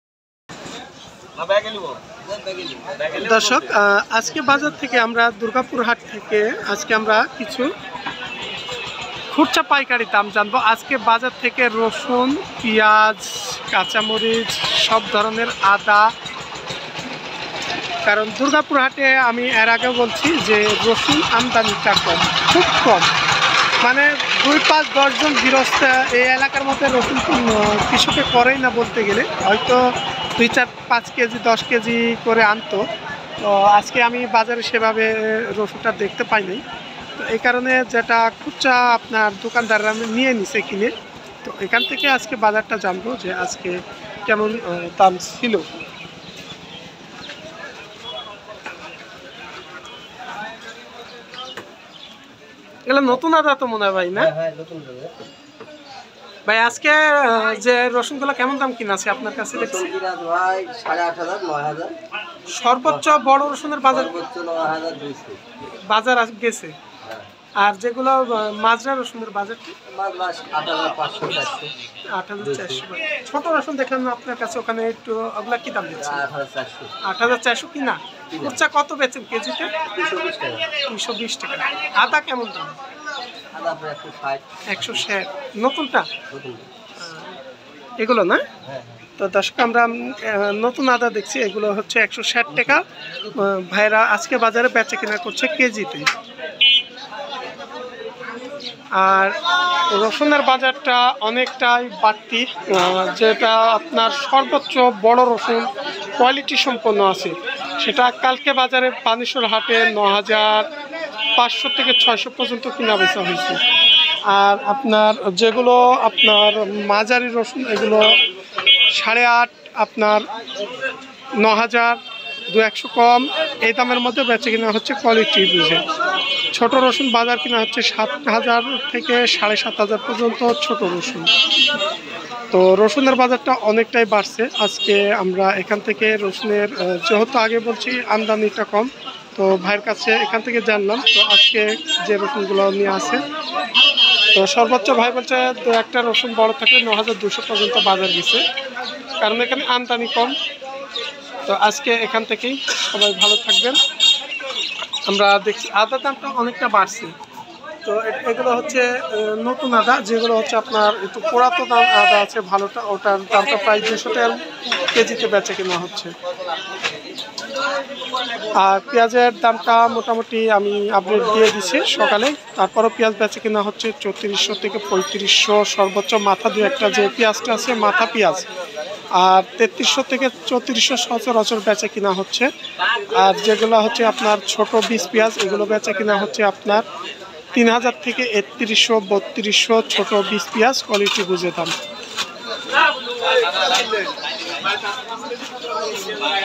11/10/2023, আজকে খবর রসুন আঁদা পেঁয়াজের বাজার দাম। রাজশাহী দূরর্গাপুর হাট। ajker bazer dor
11/10/2023, আজকে খবর রসুন আঁদা পেঁয়াজের বাজার দাম। রাজশাহী দূরর্গাপুর হাট। ajker bazer dor